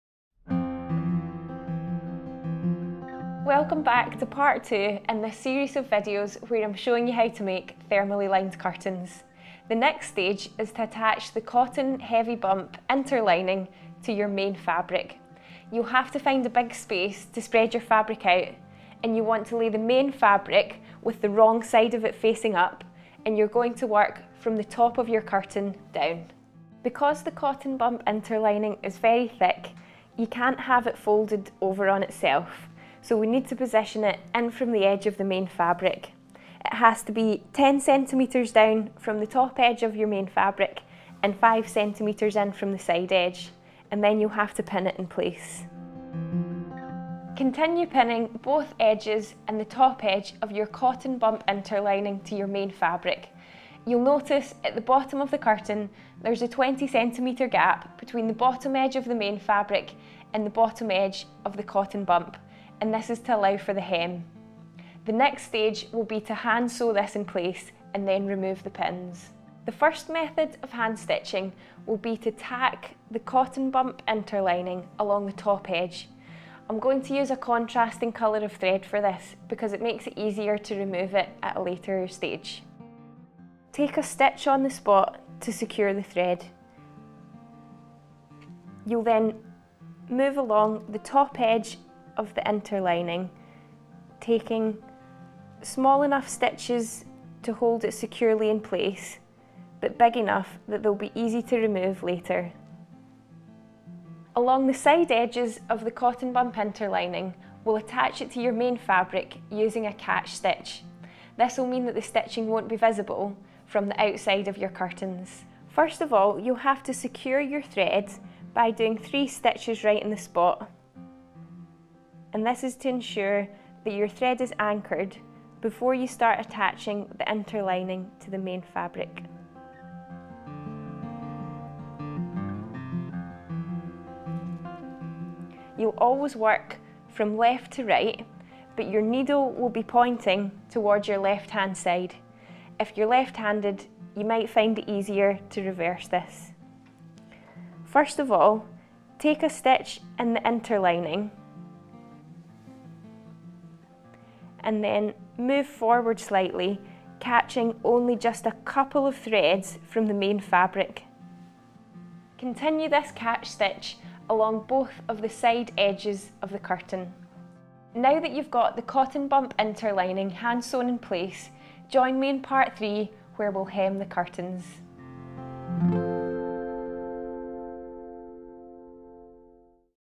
How to Make Thermally Lined Curtains - Part 2 of 5 - National Trust
Keeping your home cosy warm when it's cold outside can be difficult and expensive. One way to keep the draughts out and make your home more energy efficient is thermally lined curtains. Follow this five-part video special created by the National Trust and Good Energy to learn how to make your own thermally lined curtains. The video tutorials are presented by Lauren Guthrie, finalist of BBC2's Great British Sewing Bee, and were shot at Southwood House Farm holiday cottage at the National Trust's Calke Abbey Estate. Been inspired? Share your creation with winterwarmers and @nationaltrust This video is part 2 of 5.